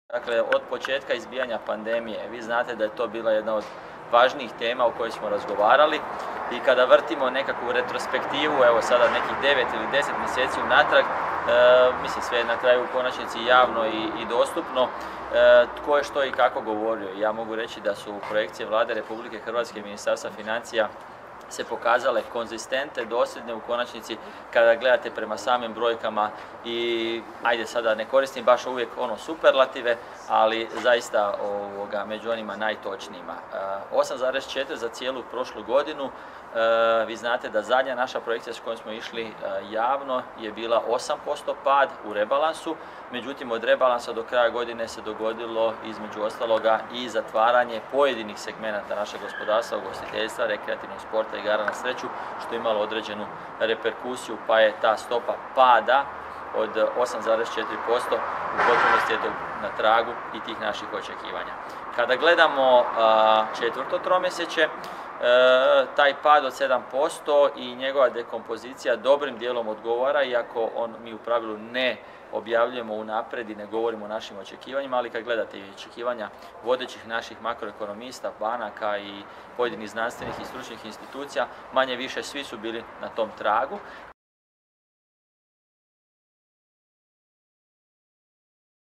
Ministar financija Zdravko Marić o padu BDP-a
Ministar financija Zlatko Marić dao izjavu novinarima oko pada BDP-a i povratu poreza.